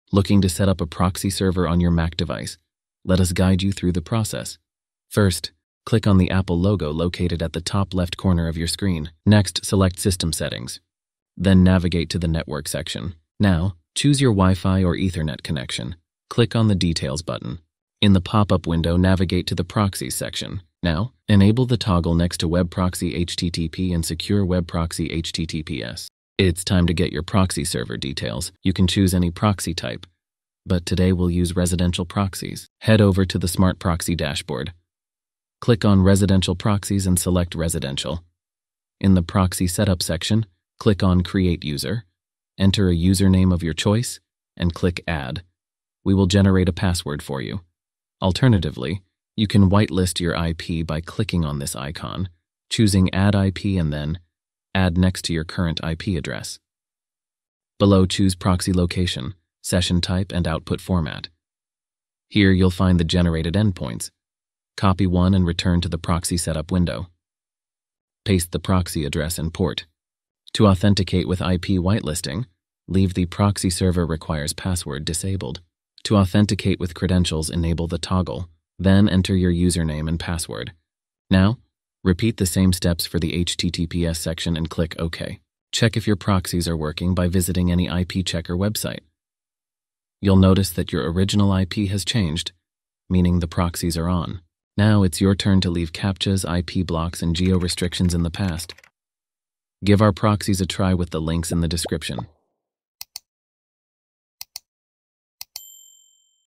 How to Configure Proxy Settings on Mac | Proxy Integration Tutorial
Looking to set up a proxy server on your Mac device? Let us guide you through the process! Tune in for the latest Decodo (formerly Smartproxy) proxy integration tutorial.

FAQs:

❓ Why use proxies with MacOS?
Proxies help protect your online activities and provide access to websites that may be restricted in specific locations. This guide will help you set up Decodo proxy services on your macOS to ensure secure and fast internet browsing.

❓ What is a proxy?
A proxy acts as an intermediary between a user and the internet. When you use a proxy server, your requests for web pages or files are processed by the proxy first. Then, the proxy forwards the request to the website. This setup allows you to mask your IP address, enhance security, access geo-restricted content, and sometimes improve speed by caching frequently accessed data.

❓ How do you configure proxies on Mac?
1. Go to the Network section on System Preferences.
2. Choose your Wi-Fi or Ethernet connection and click the Details button.
3. In the pop-up window, navigate to the Proxies section.
4. Enable the toggles next to Web Proxy (HTTP) and Secure Web Proxy (HTTPS).
5. Get your proxy details and enter them in relevant fields.
6. Test your proxies before using.

Leave CAPTCHAs, IP blocks, and geo-restrictions behind with our proxies. Try them out today.